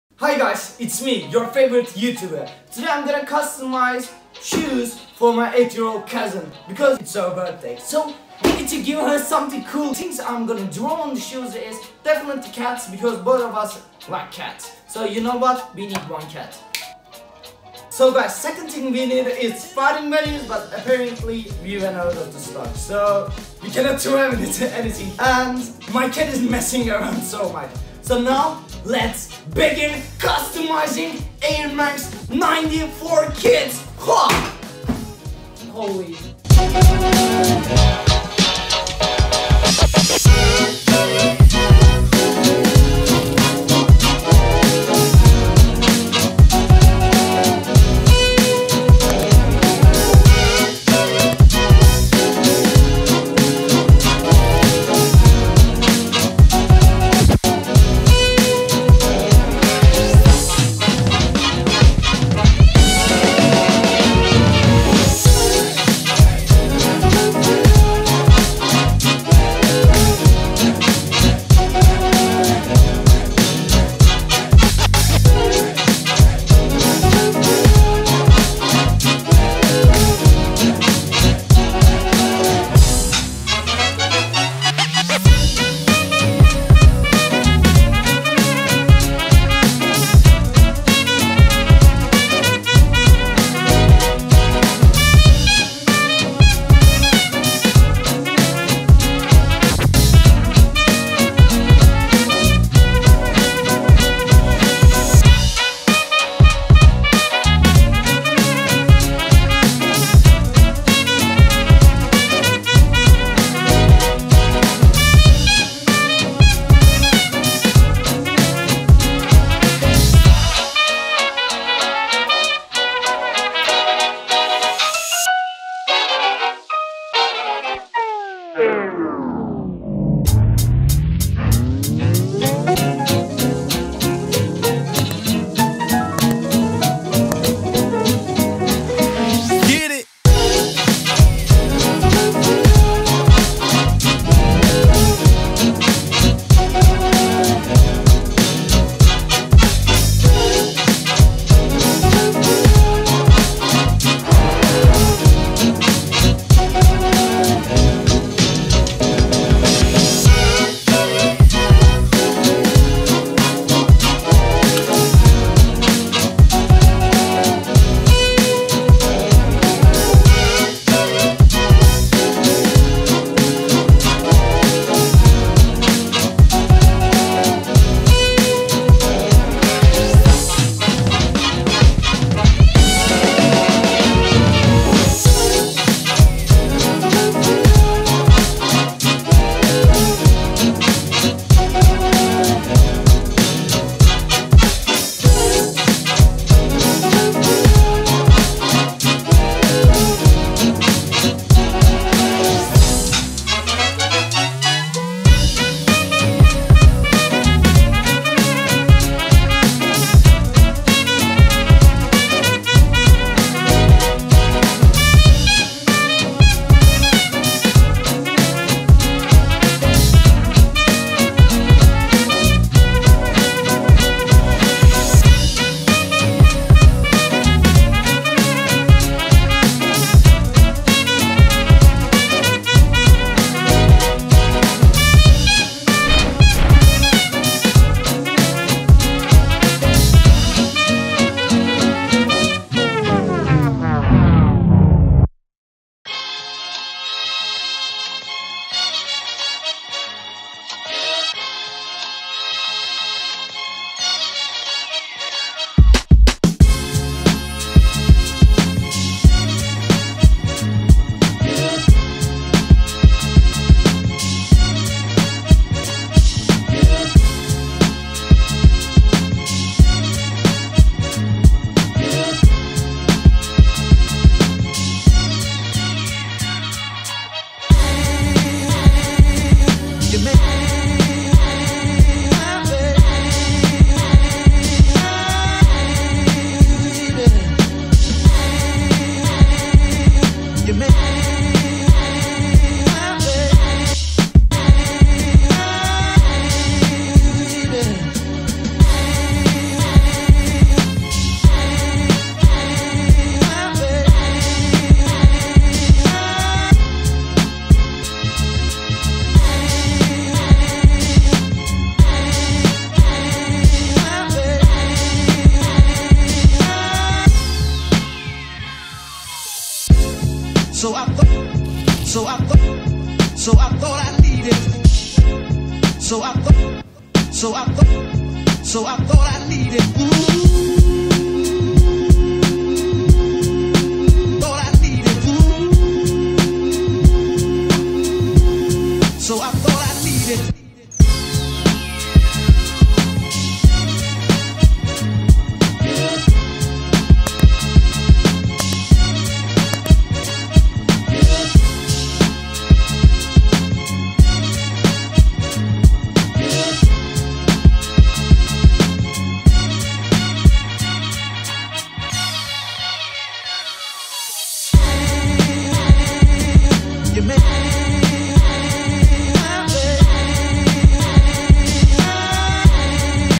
Party-colored Custom NIKE AIR MAX 90! for my LITTLE COUSIN 🎨👟 [SATISFYING]
Hey guys, In this video I custom Nike AirMax 90 shoes for my cousin as a birthday present. I hope she will like them. Let's see.